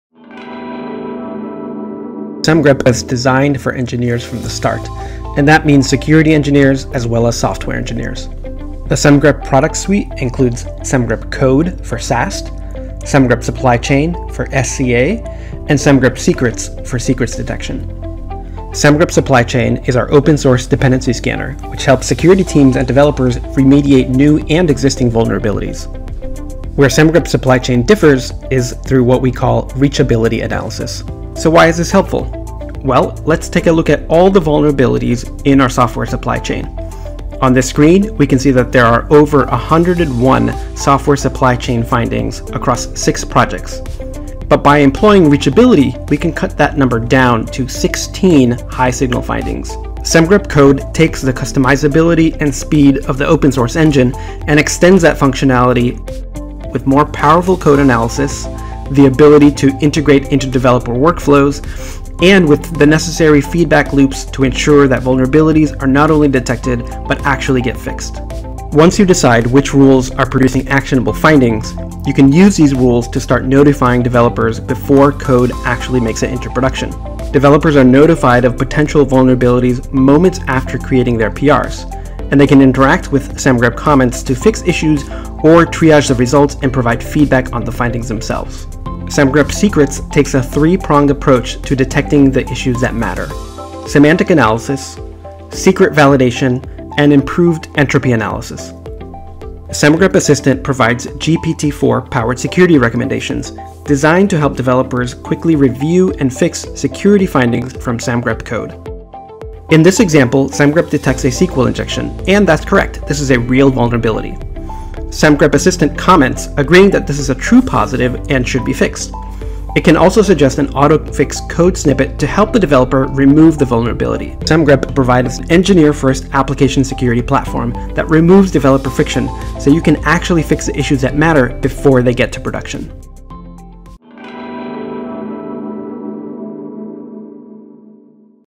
Semgrep Platform Overview
Semgrep’s application security platform is built for engineers so they can fix the issues that matter before production. Semgrep scans for vulnerabilities in your code (SAST), known supply chain risks (SCA), and accidentally committed secrets (secret scanning).